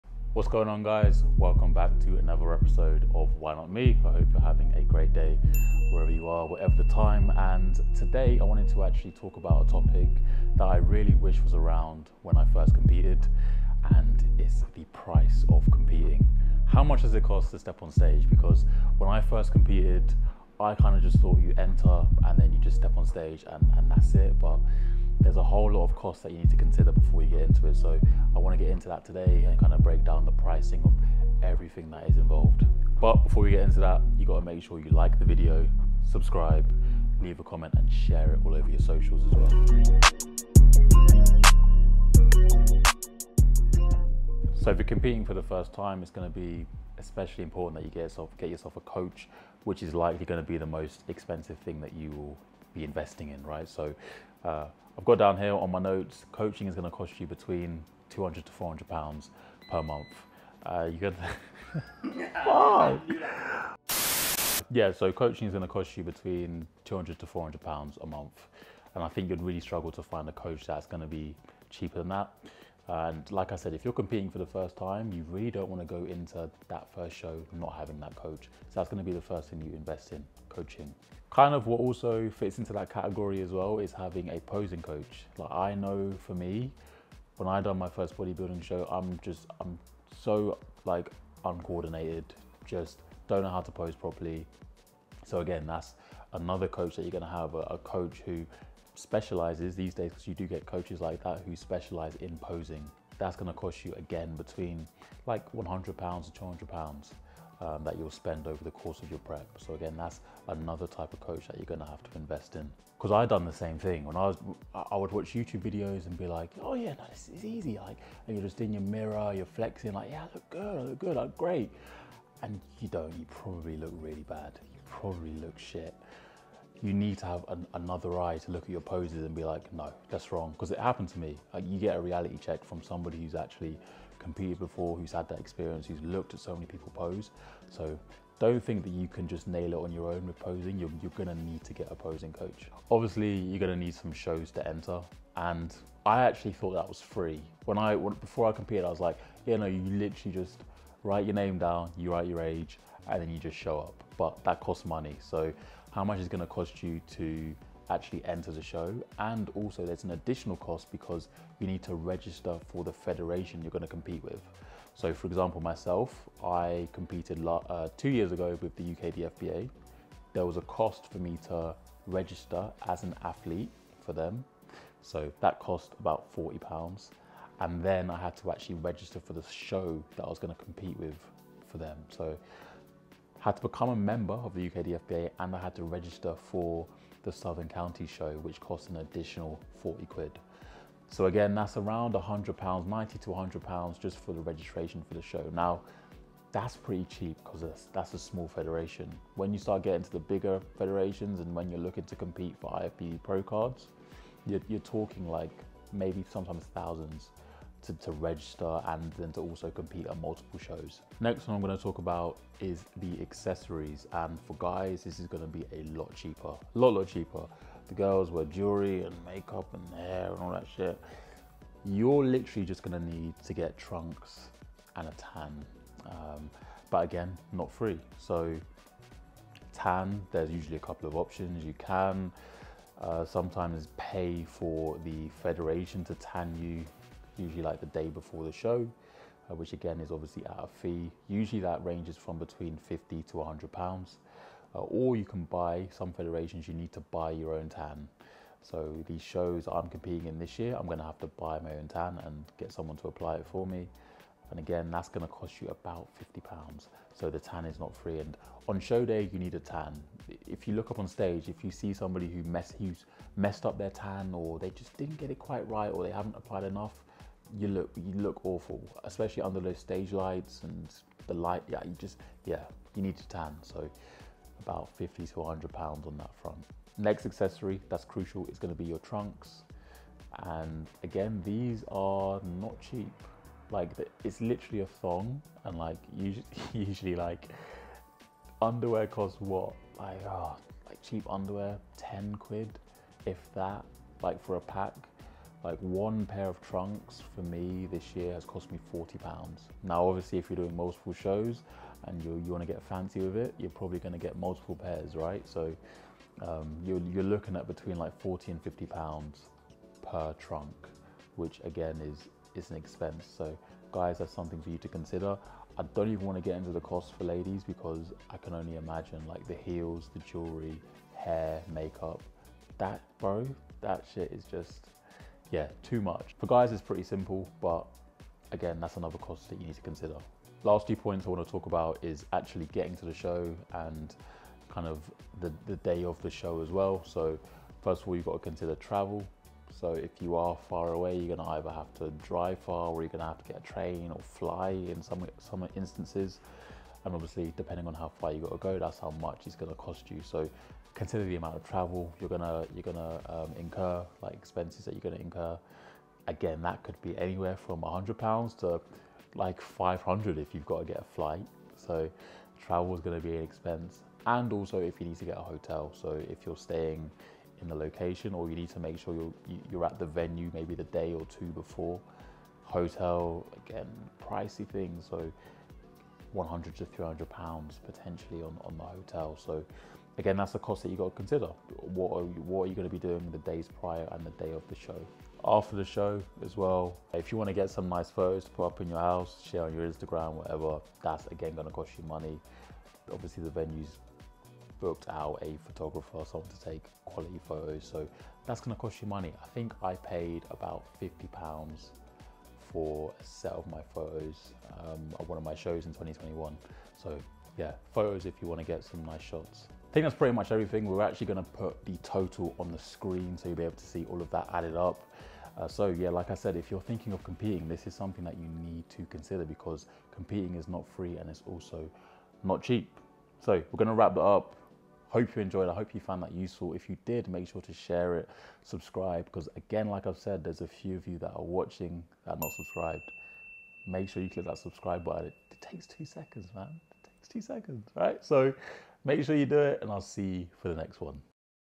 HOW MUCH DOES IT COST TO COMPETE? | MY EXPERIENCE
Let's get this one to 25 likes!

In today's video I break down the cost of competing. If you're a first time bodybuilding competitor, this is something you'll find useful.

Also, I haven't even included the cost of food and supplementation...

Really appreciate you watching the video!

For more of your boy, follow…